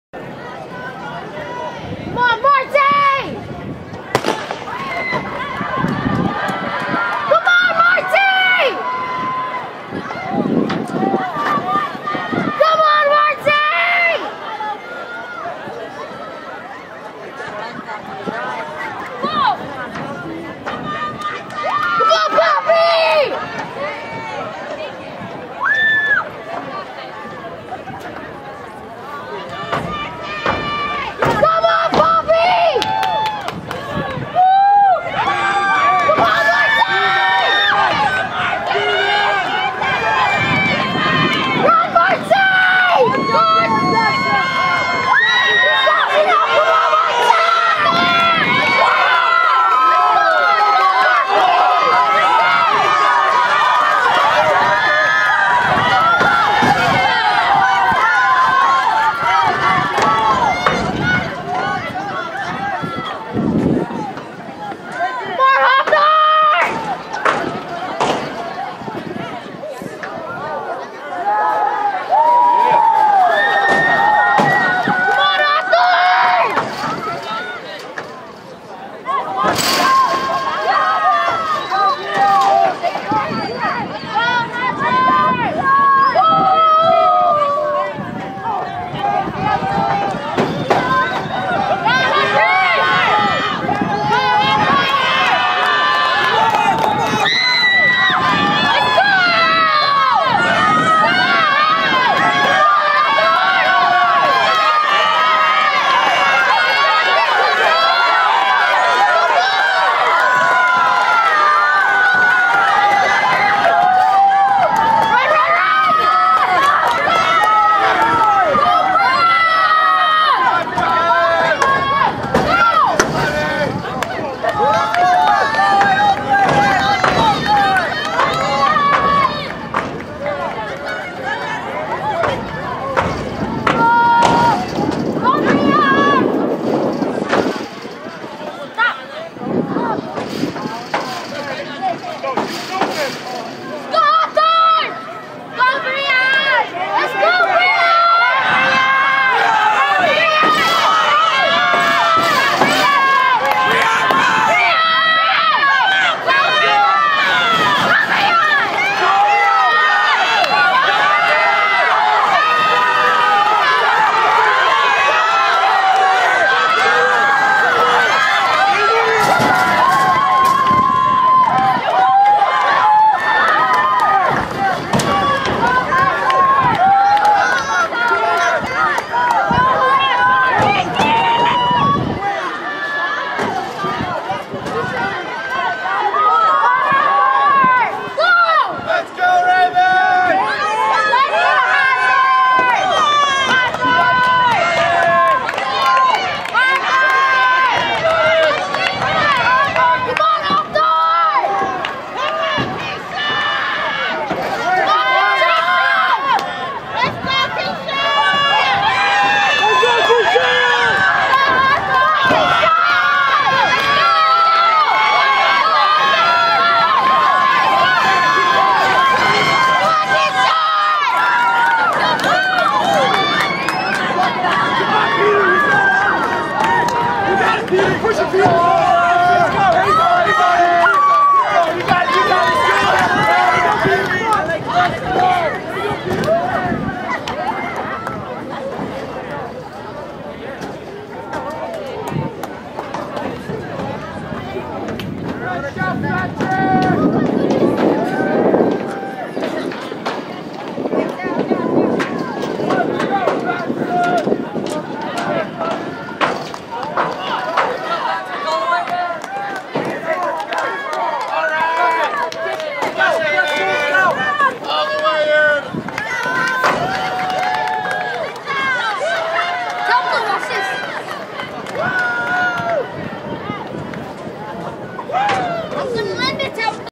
2016 El Segundo HS v Hawthorne HS Duel Track Meet April 13 Duel Track Meet March 24 B (H2) 4x400
This video show the Heat 2 of the boys' 4x400 El Segundo High School v Hawthorne High School duel track and field meet.